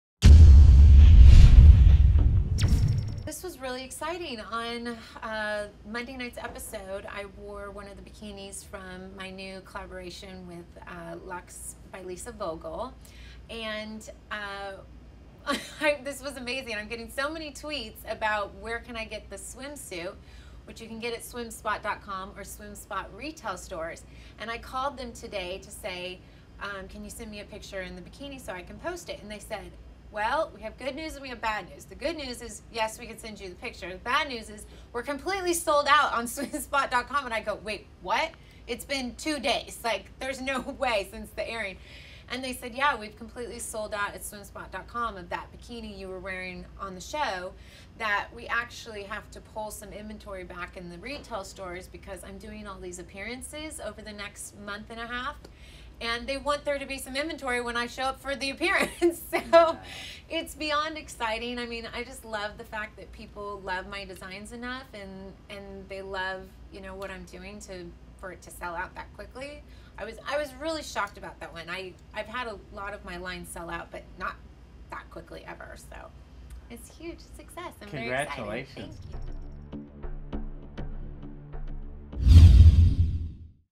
Gretchen Rossi Sells Out Of Swim Suits
After wearing one of her swimsuits on Real Housewives of Orange County, Gretchen Rossi says her swimsuits have sold out.